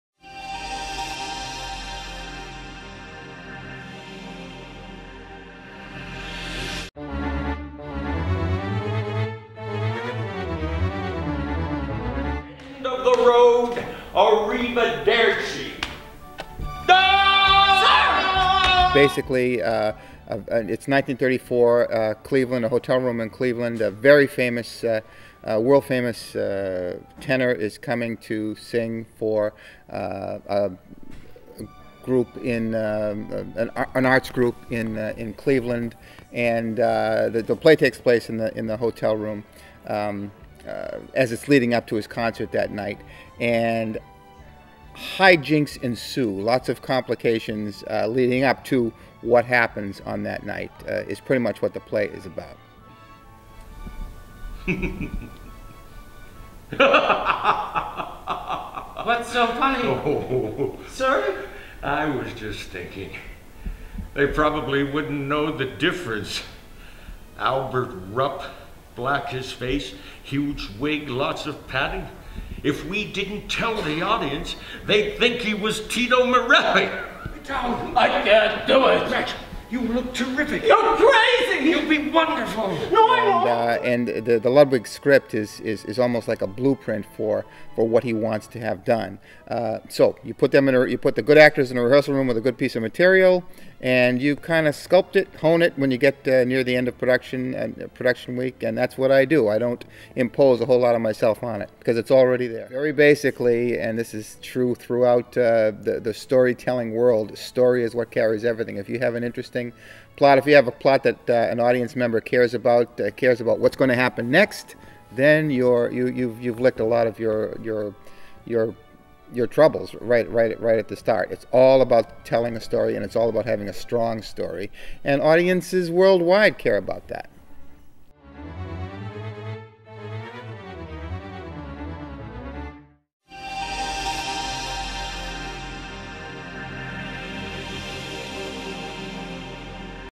Paradise City Press: New Century Theatre Presents "Lend Me a Tenor"
A brief interview with Jack Neary, director "Lend Me a Tenor", playing June 13th-22nd at New Century Theatre.